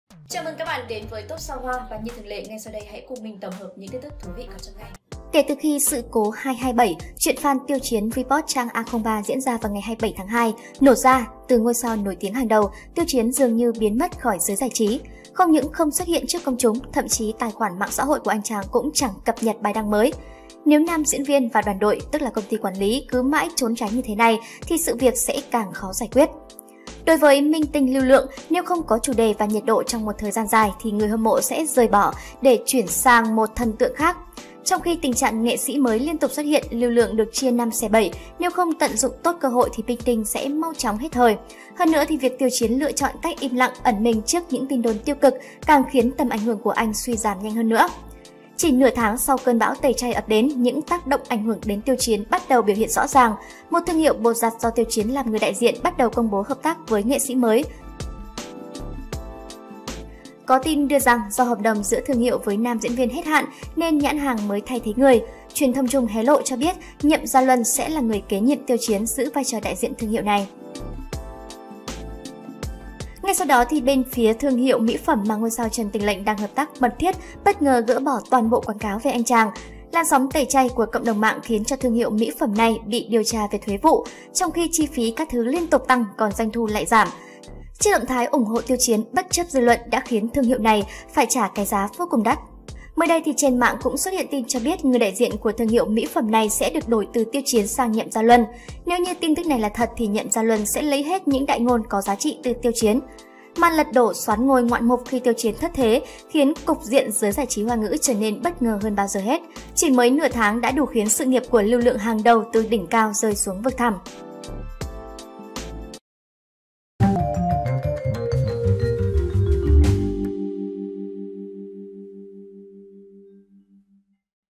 Hàng loạt thương hiệu thay thế Tiêu Chiến bằng Nhậm Gia Luân,sự nghiệp đang ở vực thẳm?
Top Sao Hoa tổng hợp tin:Hàng loạt thương hiệu thay thế Tiêu Chiến bằng Nhậm Gia Luân,sự nghiệp đang ở vực thẳm?